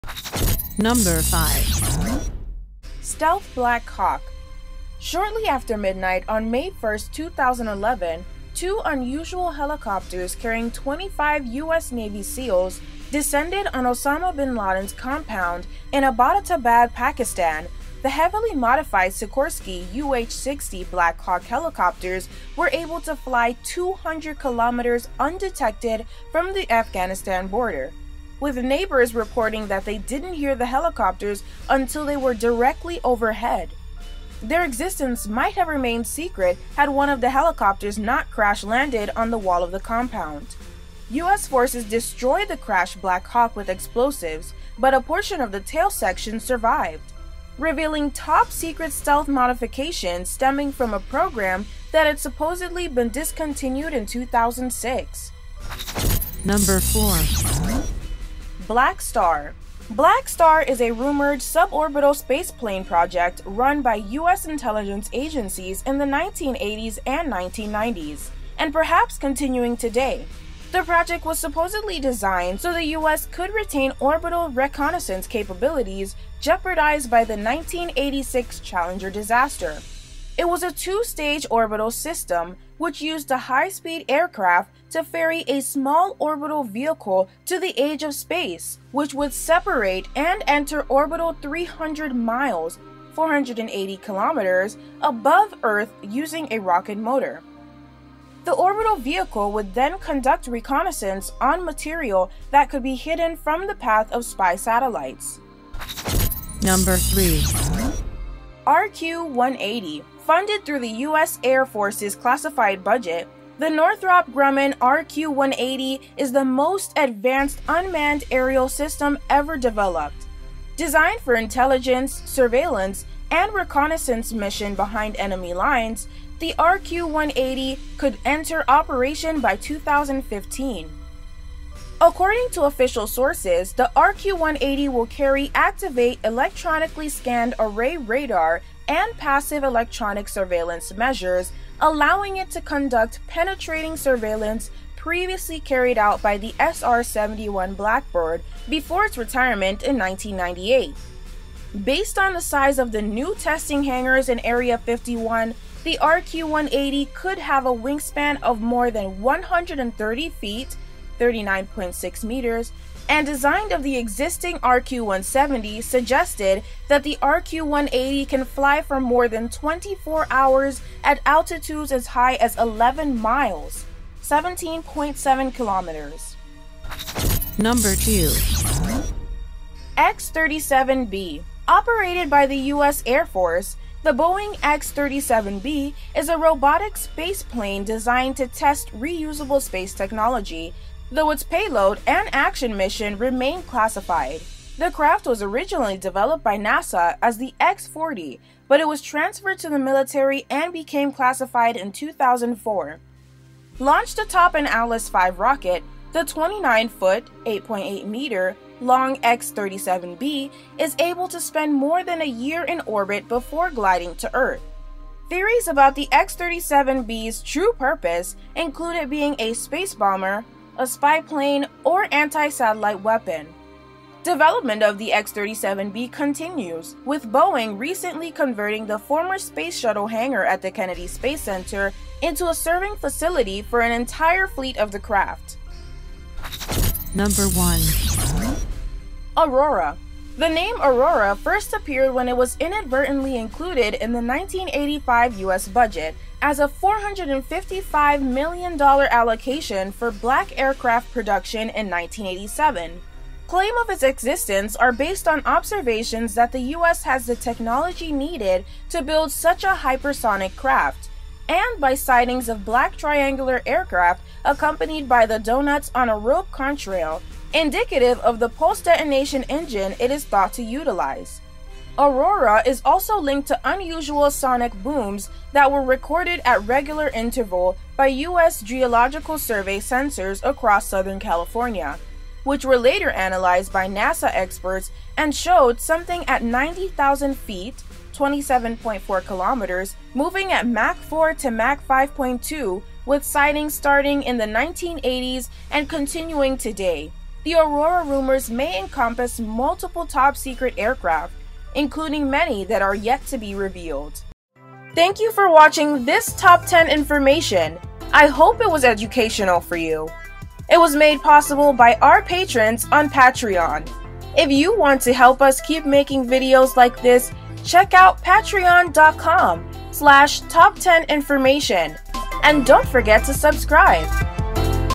5 Most Secret Military Aircraft You Don't Know
5 Most Secret Military Aircraft You Don't Know

5. STEALTH BLACKHAWK

4. BLACKSTAR

3.  RQ - 180

2. X - 37B

1. AURORA